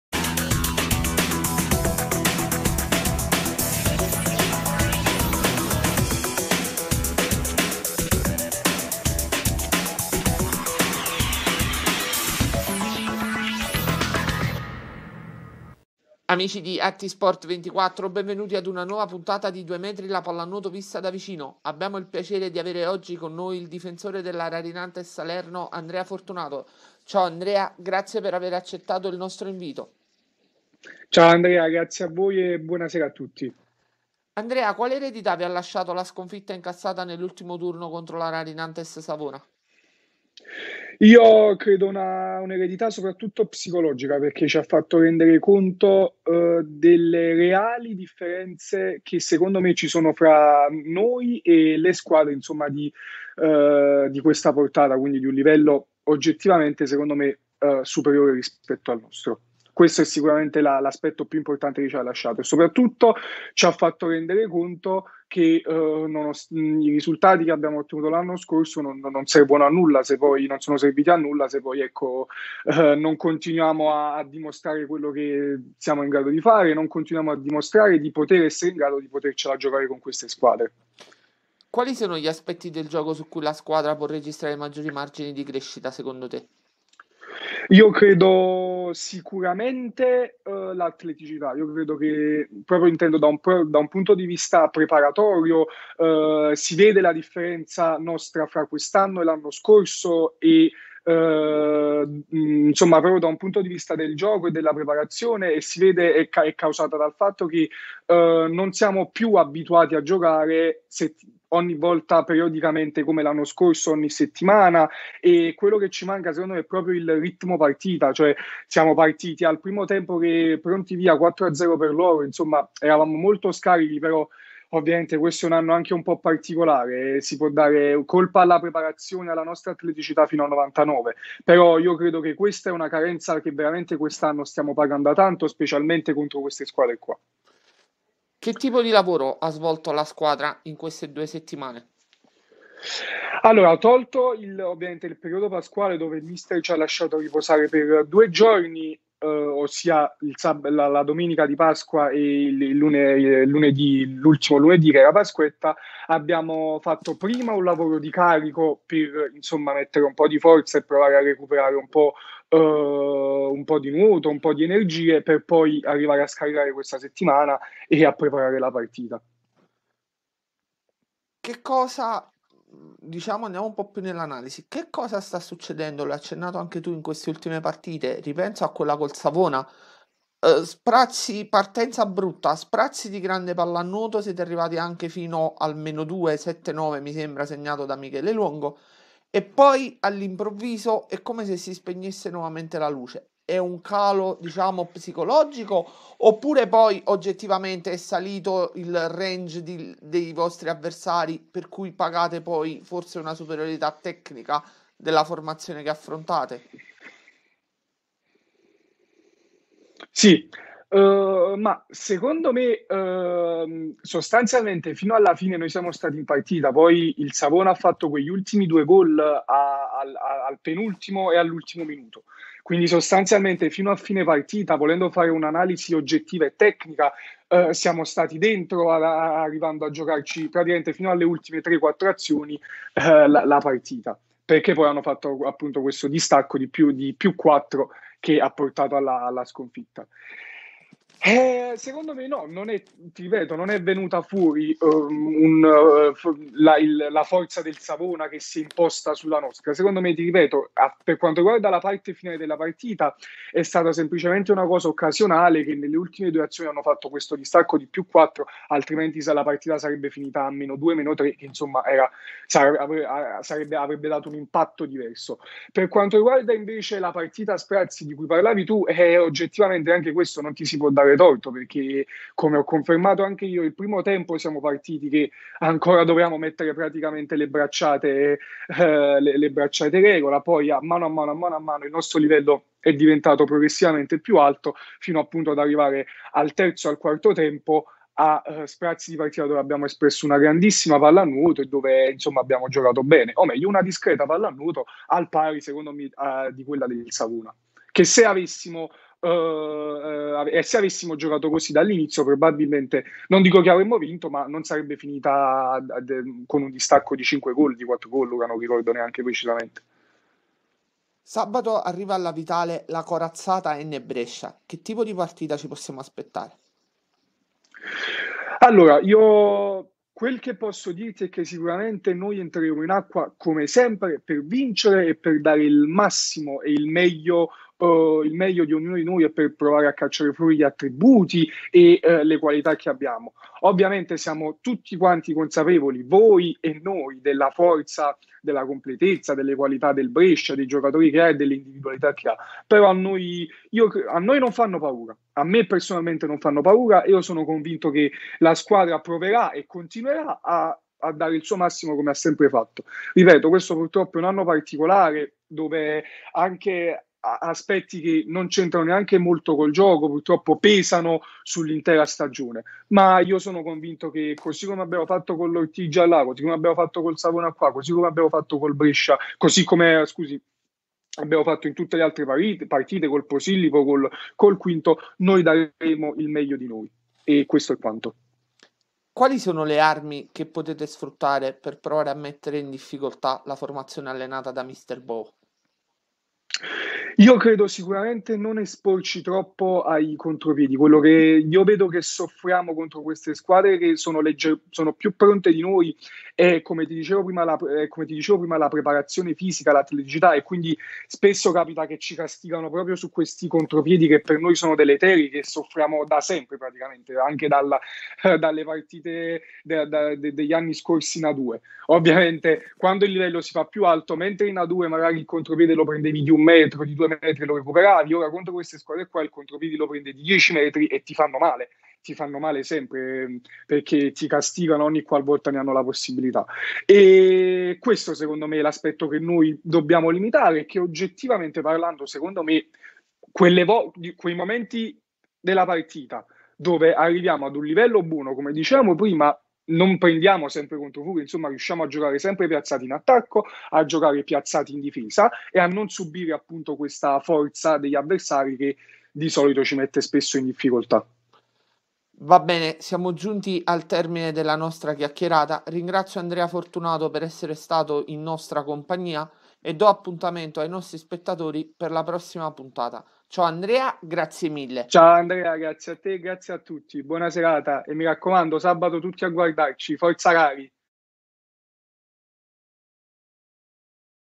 Due Metri, la pallanuoto vista da vicino 08-04-106°puntata
Andrea Fortunato, difensore della Rari Nantes Salerno, analizza il momento vissuto dalla formazione giallorossa e presenta il difficile match di sabato prossimo contro l'AN Brescia, incontro valevole per la seconda giornata del girone F del preliminary round scudetto.